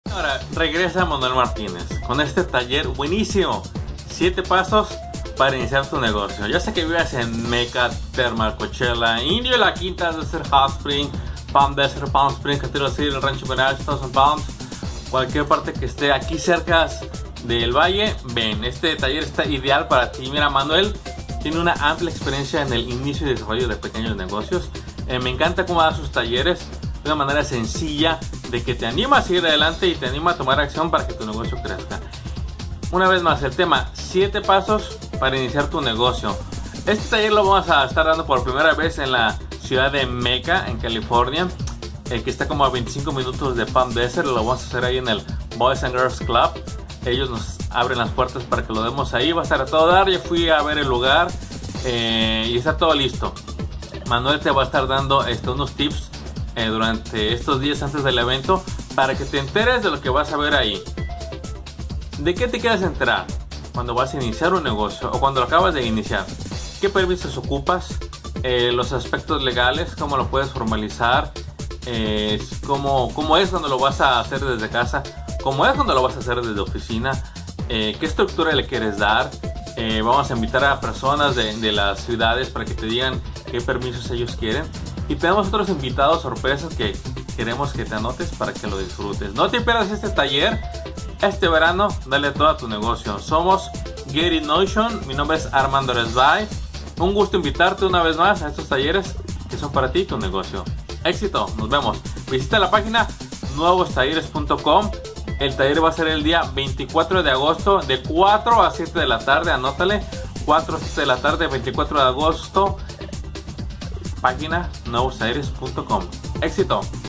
Viene taller a Mecca, CA: 7 Pasos para Iniciar tu Negocio Desde Casa u Oficina
Seminario Gratis: 7 Pasos para Iniciar tu Negocio Desde Casa u Oficina, Jueves 24 de Agosto, 4pm a 7pm, Coach Manuel Martinez, Boys and Girls Club Mecca, 91391 66th Ave, Mecca, CA, Tickets getinmotion manuelmartinez emprendedores mecca thermal coachella indio laquinta palm desert thousandpalms rancho mirage